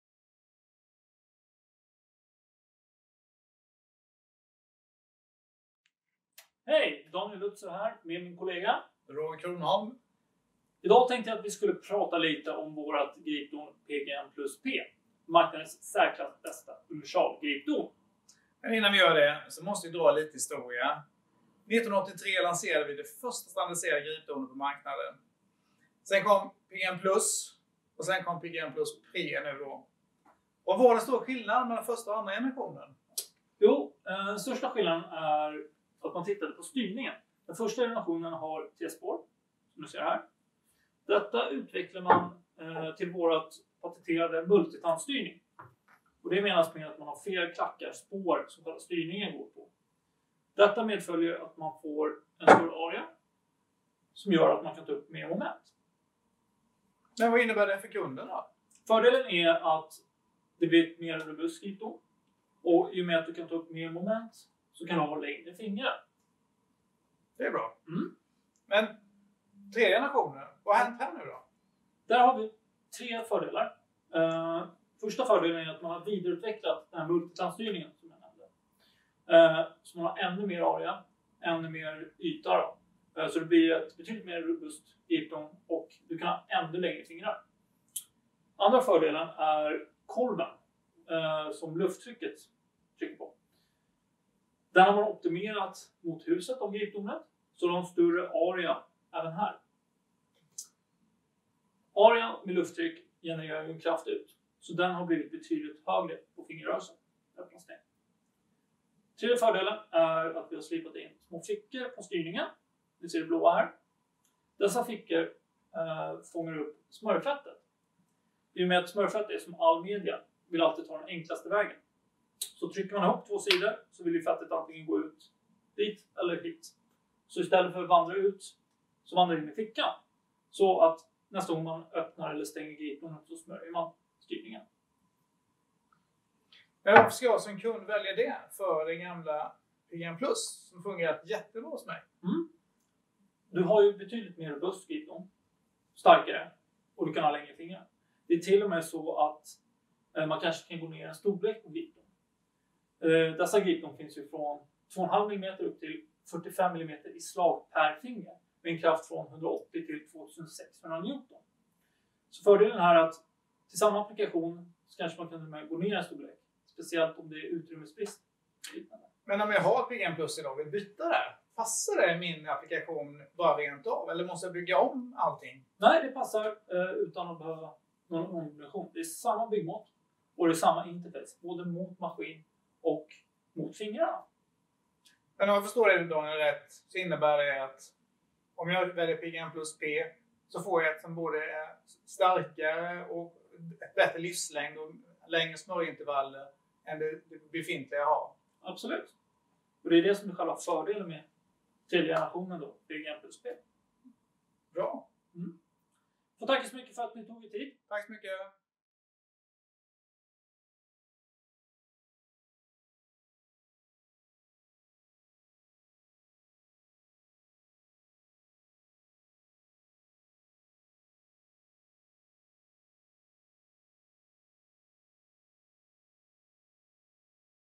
Product video of SCHUNK gripper PGN-plus-P
The SCHUNK universal gripper PGN-plus-P is the world's most proven gripper on the market. Roger Cronholm and Daniel Lutzov from SCHUNK Intec Sweden explain why the SCHUNK PGN-plus-P is so successful and point out the main features and optimizations of the new generation PGN-plus-P.